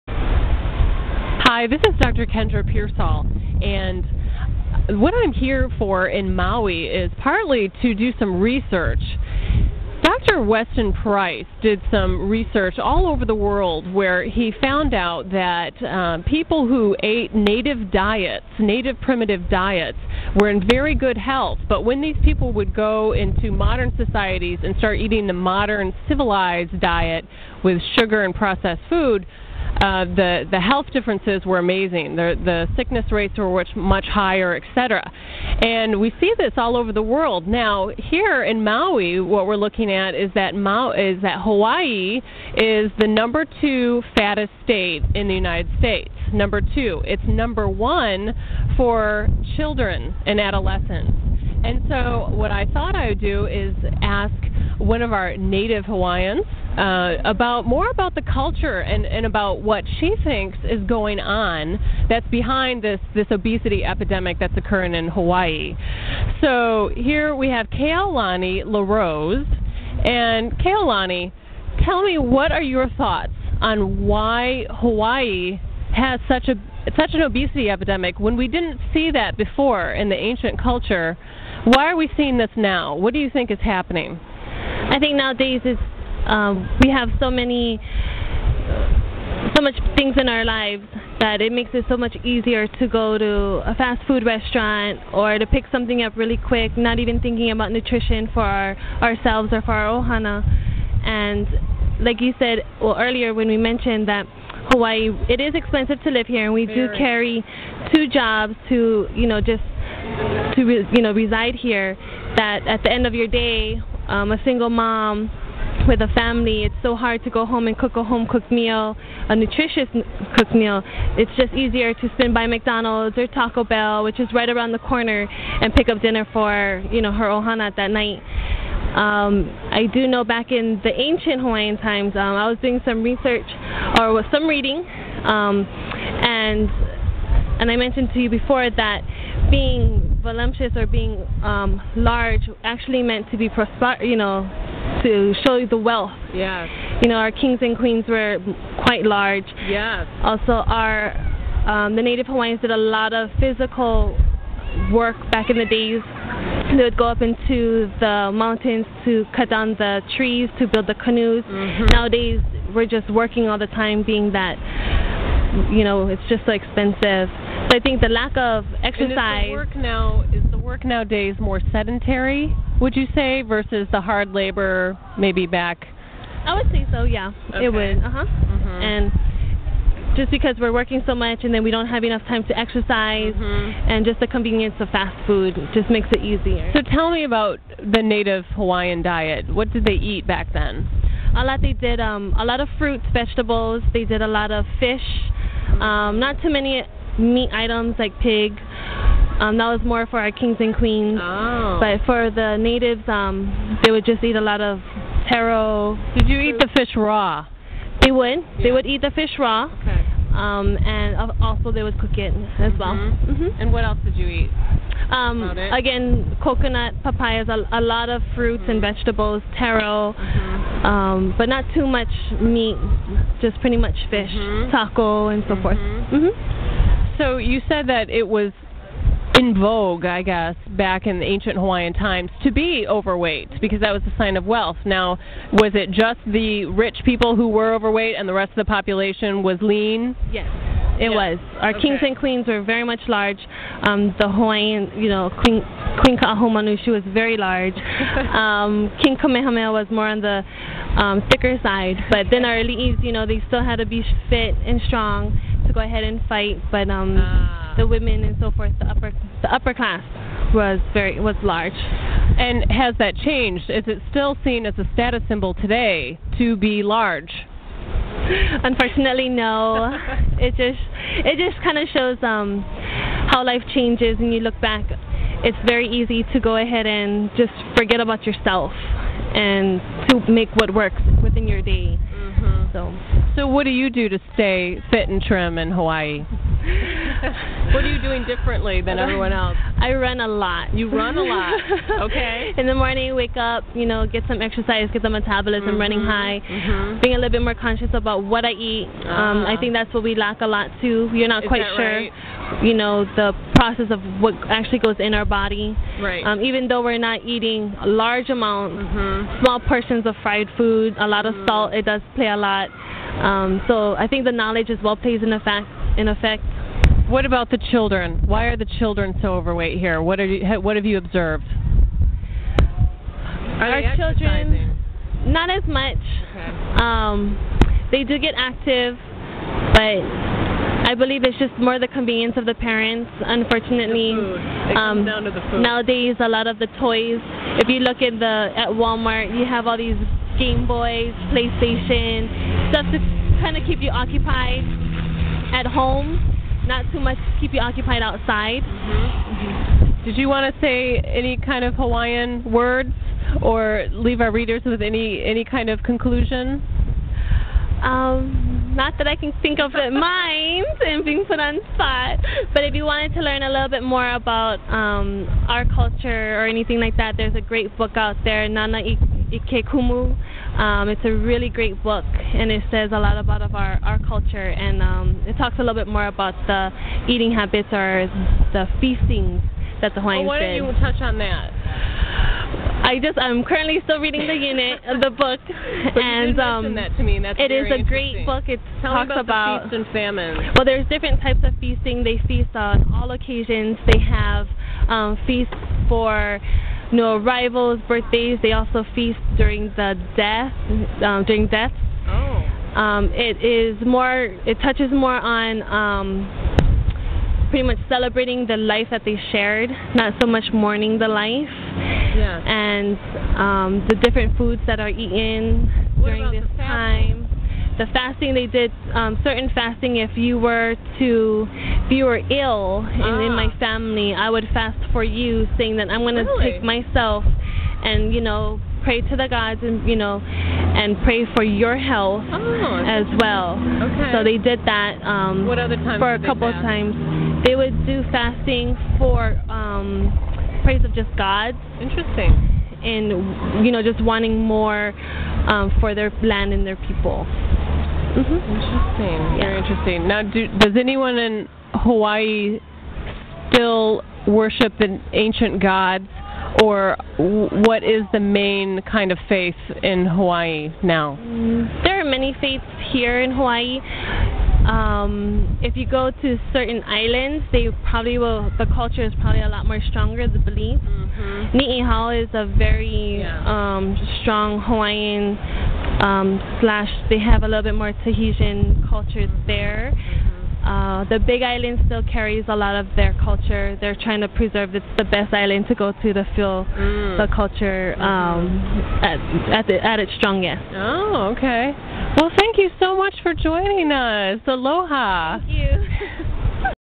Interview On Hawaiian Obesity more at
Weight loss expert Dr. Kendra Pearsall interviews Kehaulani LaRose, a native of Maui to understand how the changes in Hawaiian culture have led to an obesity epidemic.